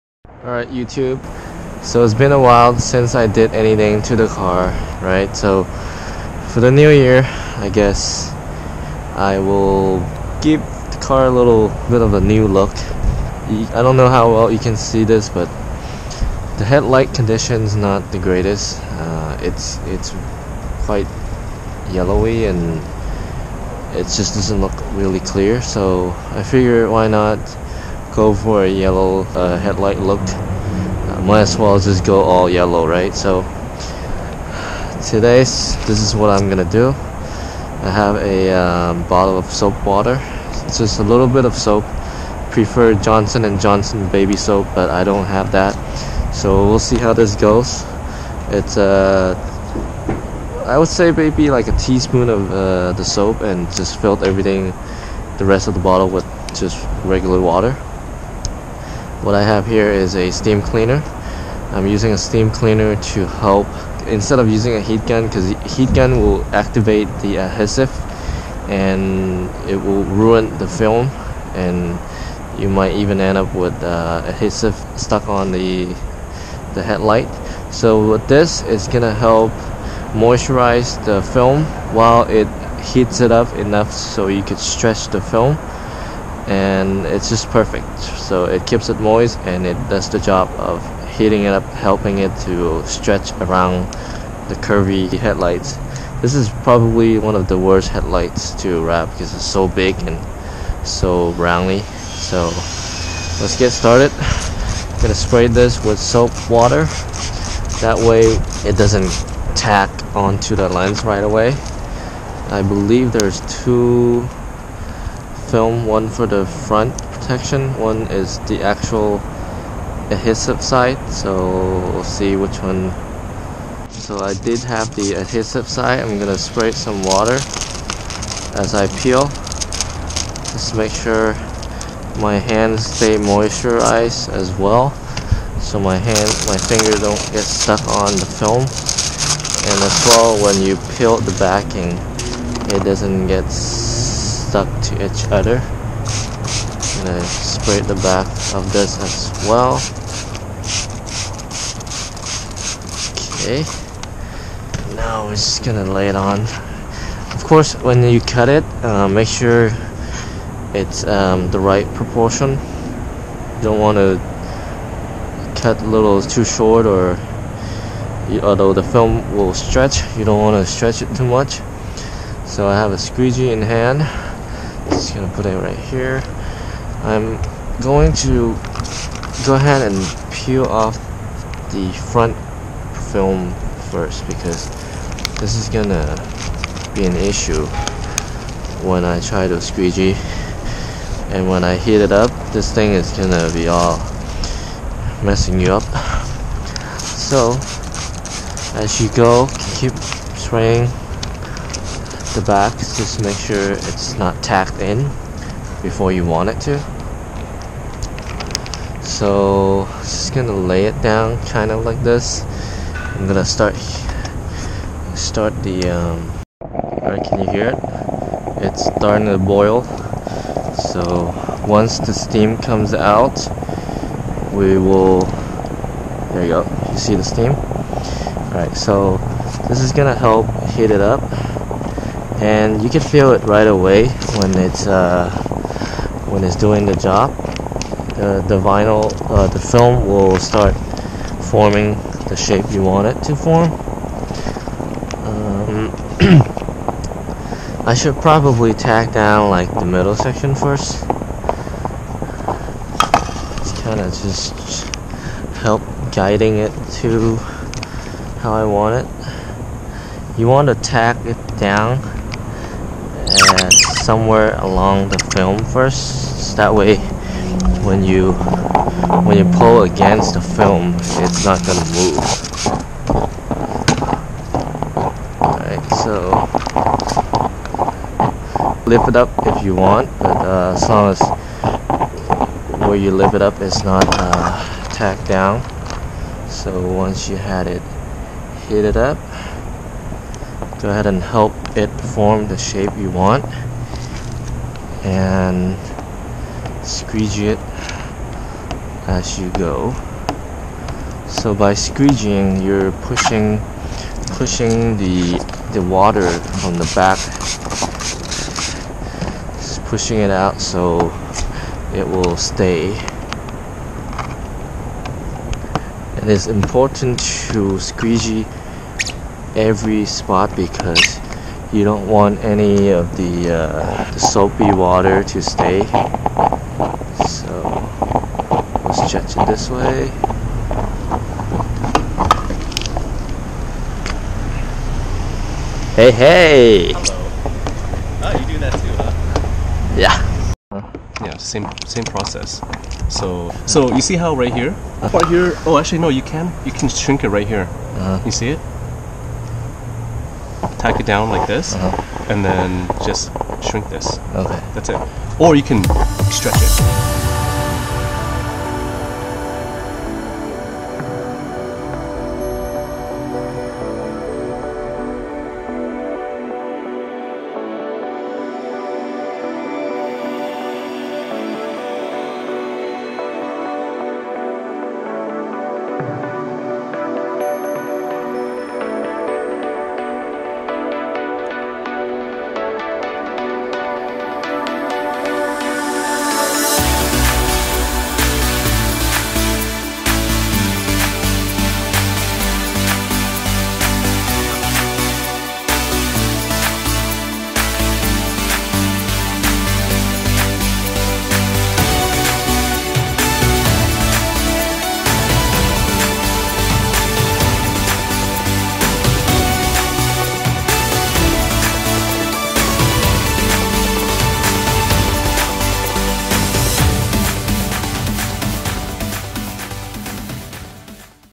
DIY amber headlight film installation MR2 Spyder MR-S
VViViD Air-Tint Extra-Wide Headlight Taillight Vinyl Tint Wrap 16" x 48" Roll Including 3M Blue Squeegee & 2x Black Felt Edge Decals (Bright Yellow)